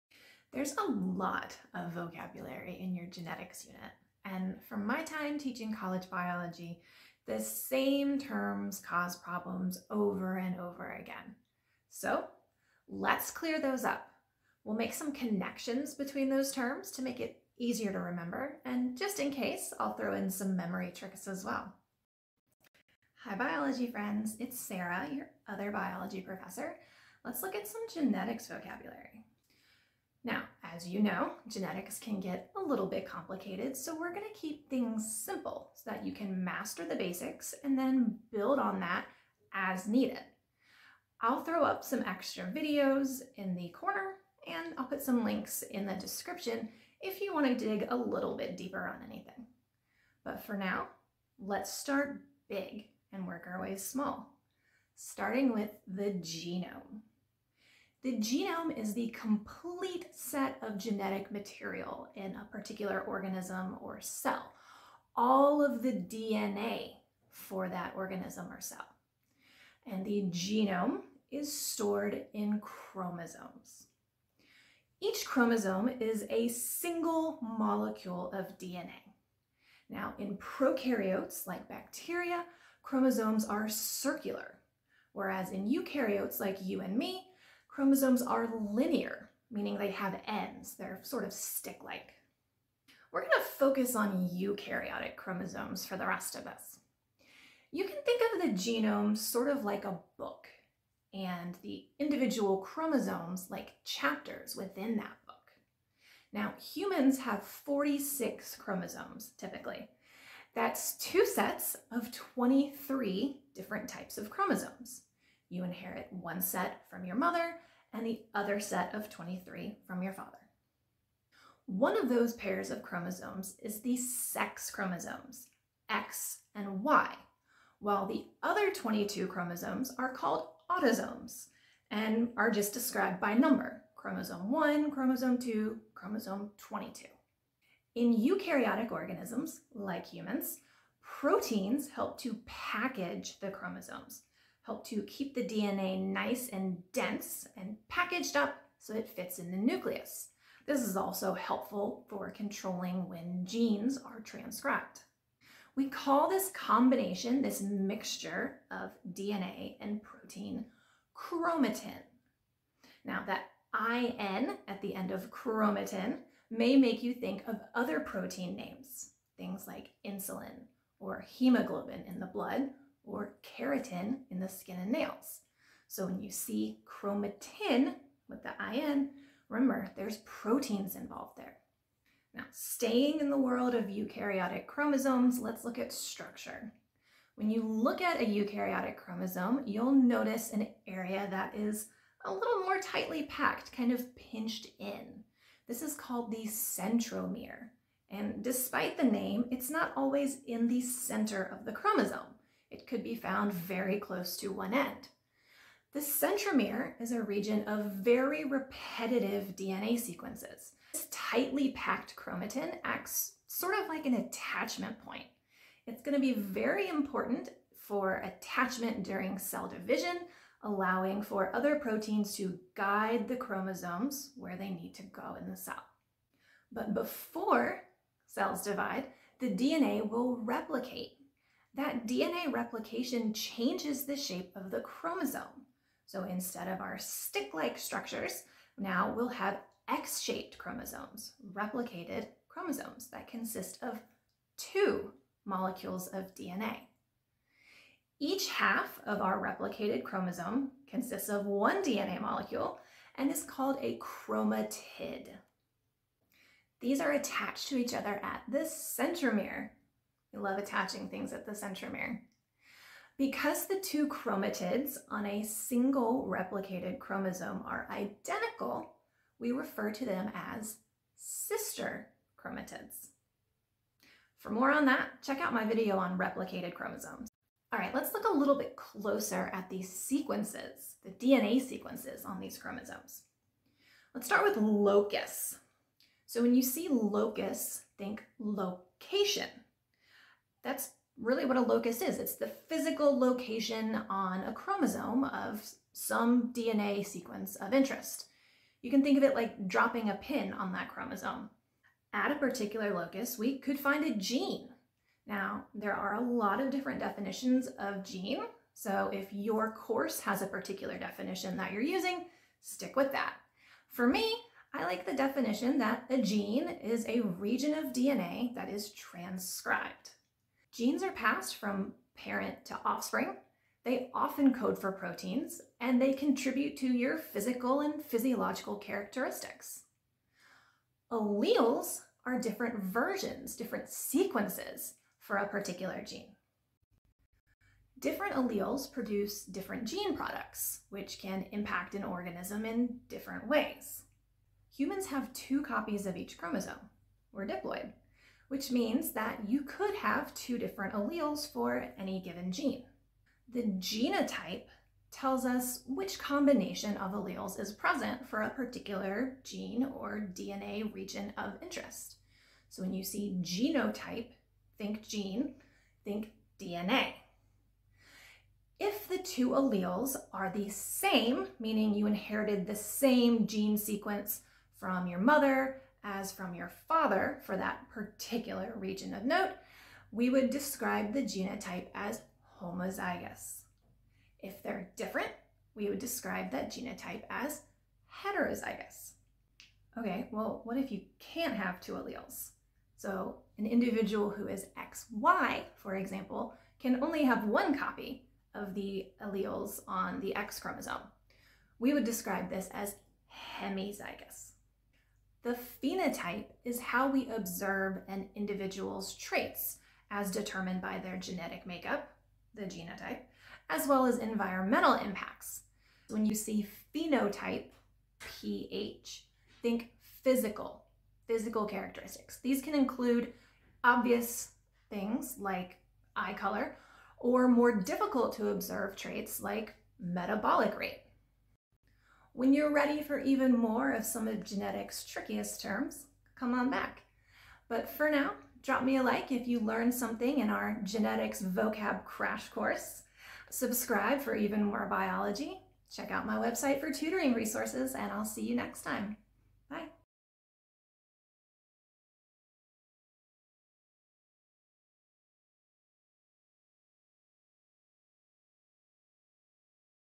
Genetics Vocabulary - A Concept Map
What are some genetics terms you NEED to know? Let's build a concept map to connect the dots on some of the most-often mixed-up genetics vocabulary, including...
Chromosome vs chromatin vs chromatid
Locus vs gene vs allele
Genotype vs phenotype
...and more!

Tutoring information AND a blank version of this concept map (for practice!)

00:00 Intro
00:57 GENOME vs CHROMOSOME
01:13 Prokaryotic vs. eukaryotic chromosomes
01:36 AUTOSOME vs SEX CHROMOSOME
02:17 CHROMATIN
03:08 CENTROMERE
03:46 CHROMATID (REPLICATED CHROMOSOMES and SISTER CHROMATIDS)
04:37 LOCUS (and loci)
05:03 GENE
05:30 ALLELE
05:44 GENOTYPE
06:08 HOMOZYGOUS, HETEROZYGOUS, and HEMIZYGOUS
06:45 PHENOTYPE
07:11 Genetics vocabulary concept map

The GENOME is the complete set of genetic material in a particular organism or cell - all of the DNA for that organism or cell.
And the genome is stored in CHROMOSOMES.
Each chromosome is a single molecule of DNA.
Now, in prokaryotes like bacteria, chromosomes are circular whereas in eukaryotes like you and me, chromosomes are linear, meaning they have ends.

In eukaryotic organisms like humans, proteins help to package the chromosomes, help to keep the DNA nice and dense and packaged up so it fits in the nucleus.
This is also helpful for controlling when genes are transcribed.
We call this combination this mixture of DNA and protein CHROMATIN.

When you look at a eukaryotic chromosome you'll notice an area that is a little more tightly packed, kind of pinched-in.
This is called the CENTROMERE, and despite the name it's not always in the center of the chromosome.
The centromere is a region of very repetitive DNA sequences.
This tightly packed chromatin acts sort of like an attachment point, it's going to be very important for attachment during cell division, allowing for other proteins to guide the chromosomes where they need to go in the cell.

DNA replication changes the shape of the chromosome so instead of our stick-like structures, now we'll have x-shaped chromosomes - replicated chromosomes that consist of two molecules of DNA.
Each half of our replicated chromosome consists of one DNA molecule and is called a CHROMATID.
These are attached to each other at the centromere.
(We love attaching things at the centromere.)
Because the two chromatids on a single replicated chromosome are identical, we were refer to them as SISTER CHROMATIDS.

So when you see "LOCUS," think "location."
That's really what a locus is, it's the physical location on a chromosome of some DNA sequence of interest.

At a particular locus we could find a GENE.
Genes are passed from parent to offspring, they often code for proteins, and they contribute to your physical and physiological characteristics.

ALLELES are different versions, different sequences for a particular gene.
Different alleles produce different gene products, which can impact an organism in different ways.

The GENOTYPE tells us which combination of alleles is present for a particular gene or DNA region of interest.

If the two alleles are the same (you inherited the same gene sequence from your mother as from your father), we would describe the genotype as HOMOZYGOUS.
If they're different we would describe that genotype as HETEROZYGOUS.
Okay, well... what if you can't have two alleles?
An individual who is XY for example can only have one copy of the alleles on the X chromosome - we would describe this as HEMIZYGOUS.

The PHENOTYPE is how we observe an individual's traits (physical, physiological, behavioral) as determined by their genetic makeup (the genotype) as well as environmental impacts.